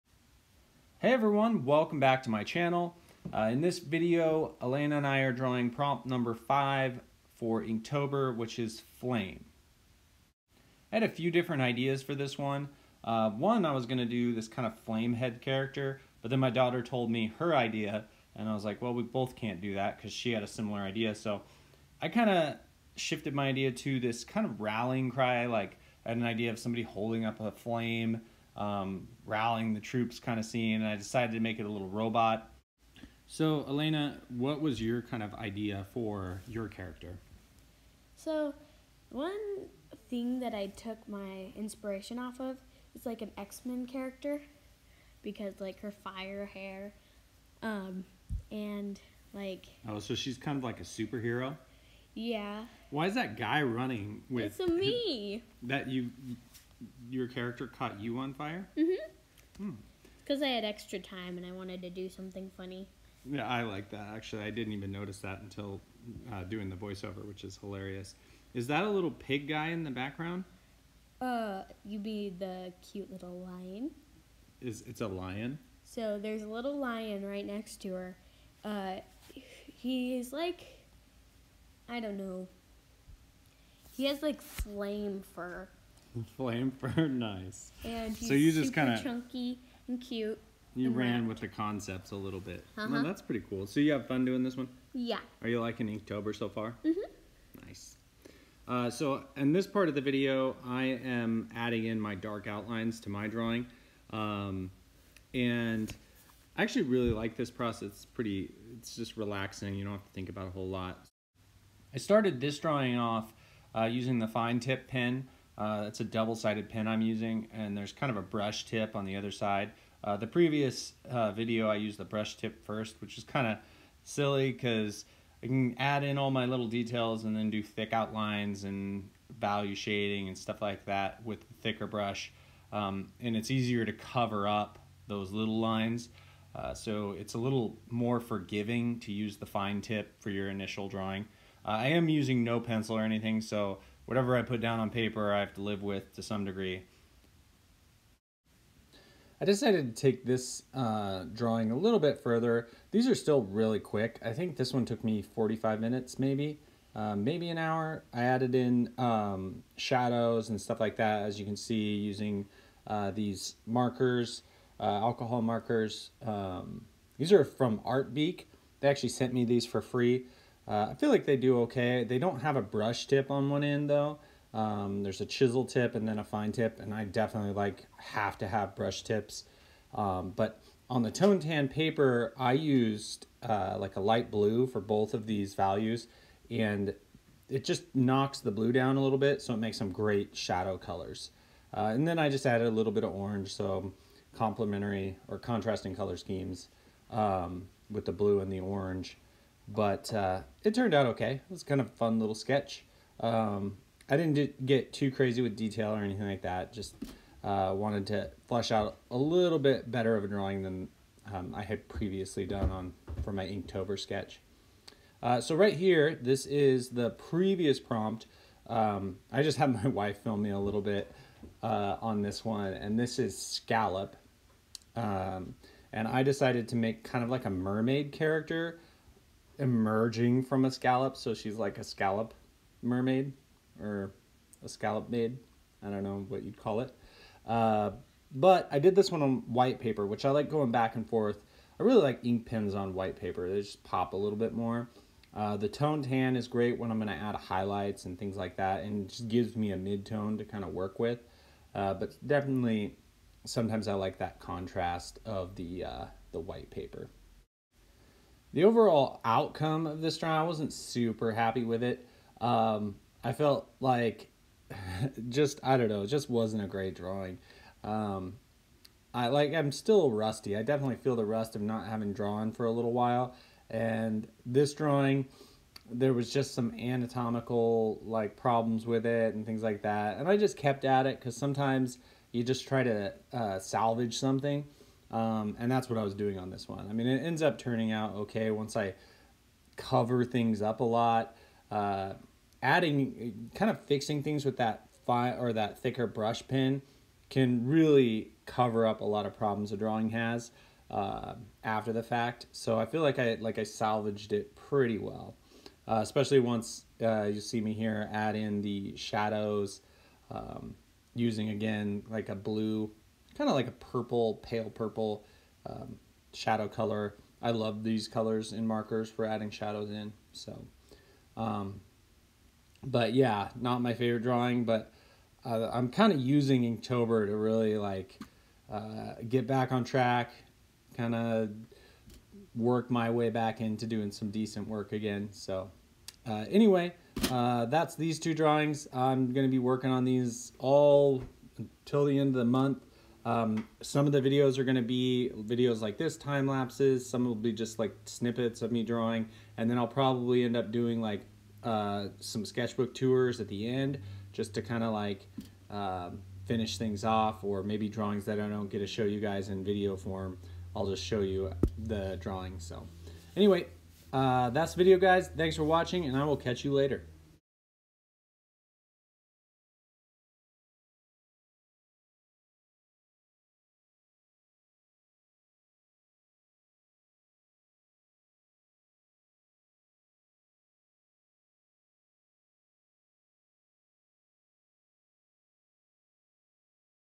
Inktober 4 Scallop and 5 Flame
In this video, Alaina and I draw the Inktober prompt #5 Flame and I draw Inktober Prompt #4 Scallop I used ink pen and markers to draw my Robot leader on strathmore toned tan paper. Alaina drew a Flame head girl using pencil and ink on white paper. the drawings took about 45 minutes to complete.

Trav and Alaina